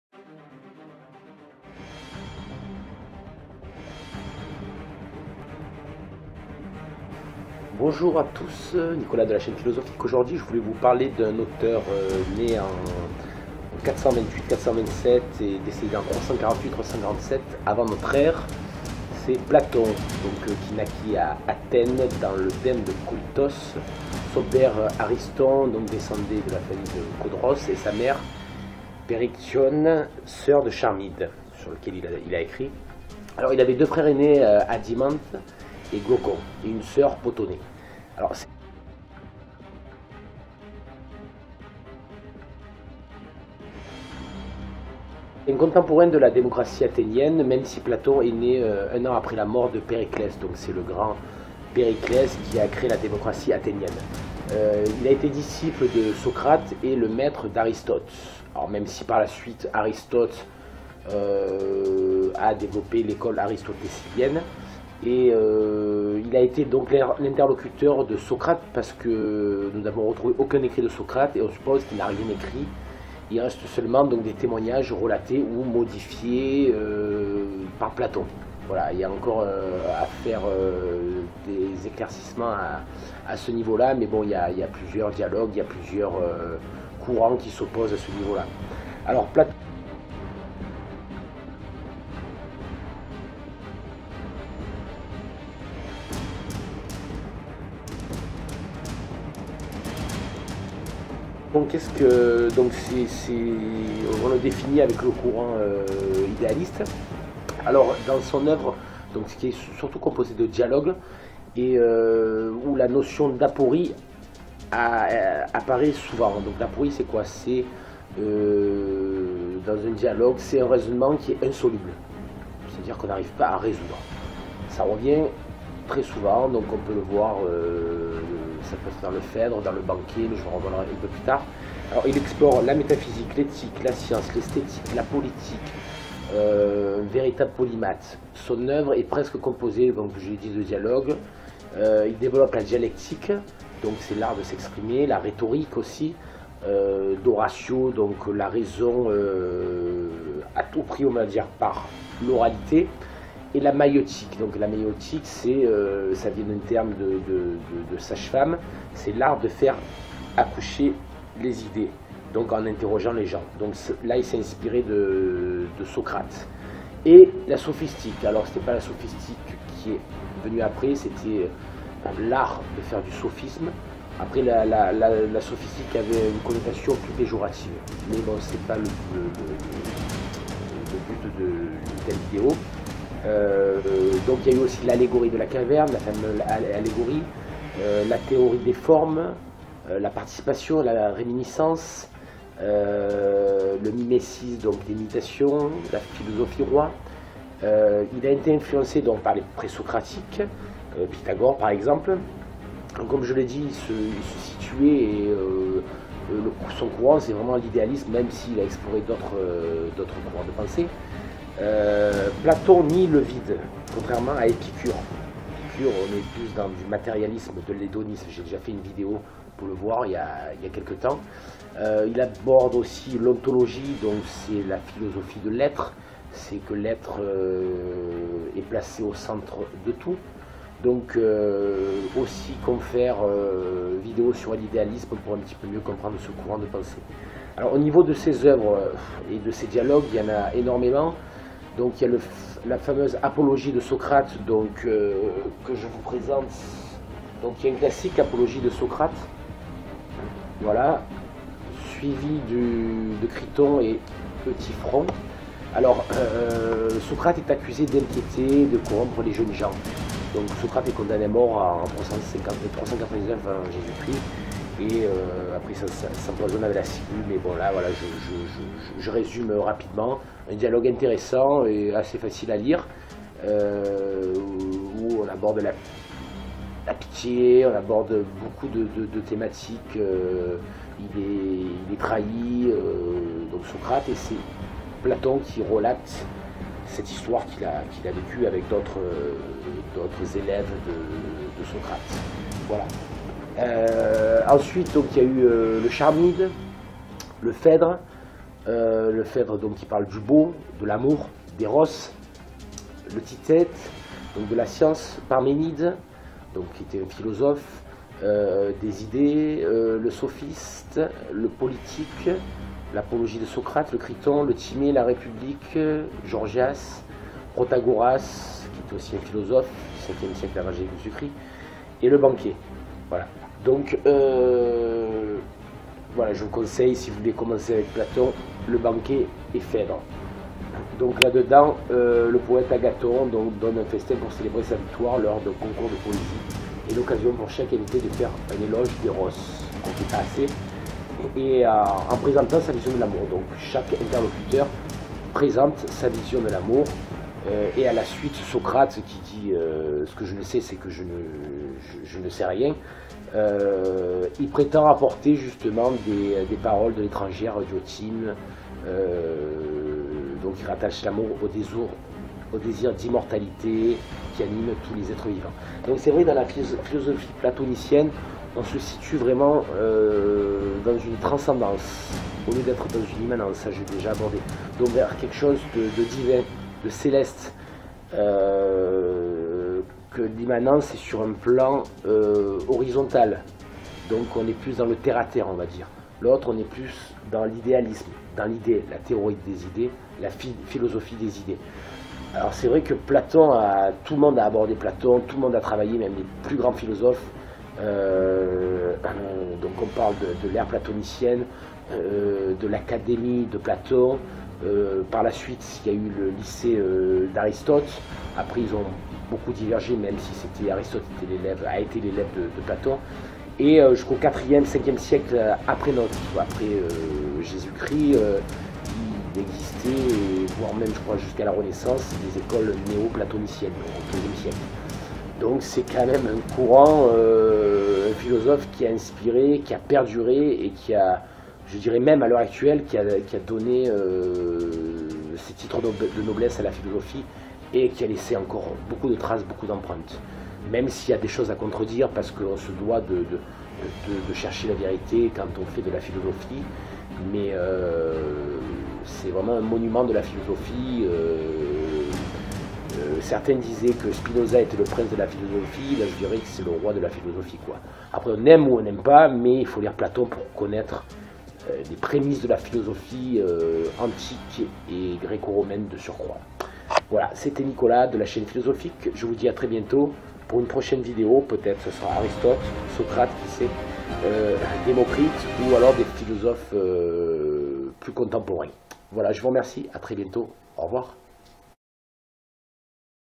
PLATON : L' IDÉALISTE
PLATON : L' IDÉALISTE (-428/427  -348/347  avant notre ère)
naquit à Athènes dans le dème de Collytos , son père Ariston descendait de Codros et sa mère Périctionè sœur de Charmide.  Il avait 2 frères ainés Adimante et Glaucon, 1 sœur Potonè.

Contemporain de la démocratie athénienne même si Platon né 1 an après la Mort de Périclès.
Disciple de Socrate, il a été maître d'Aristote
Il a été l'interlocuteur de Socrate pour lequel nous n'avons retrouvé aucun écrit. On suppose qu'il n'a rien écrit et il reste seulement des témoignages relatés ou modifiés par Platon.

Afin de me permettre de faire évoluer cette chaine. Merci par avance